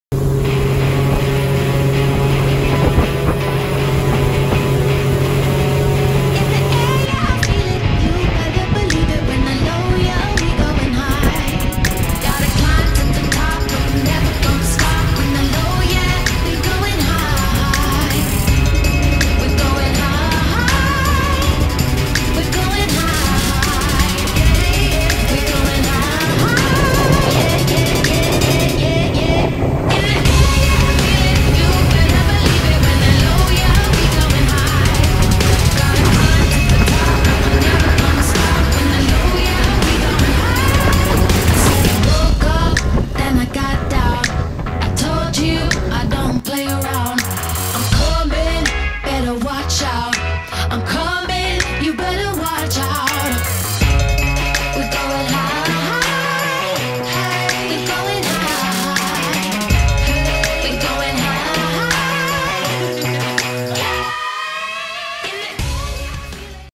The New Wildcat
The all new Wildcat combines a Thundercat style racing hull with tandem seating and joy stick steering .. for the most thrilling and agile craft on the water.  Visit wildcatboats.com to learn more.